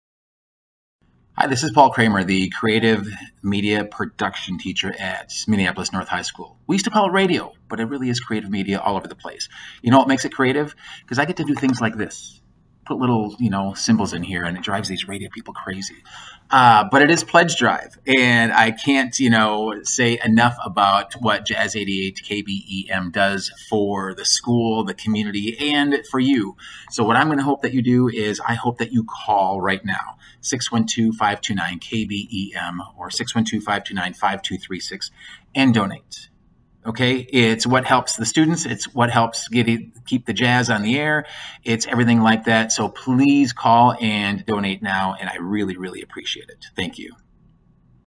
Paul Kraimer talks about the Jazz88 Fall Member Drive
Paul is the Creative Media Productions teacher at Mpls. North High School and he explains why your donation is important to his students.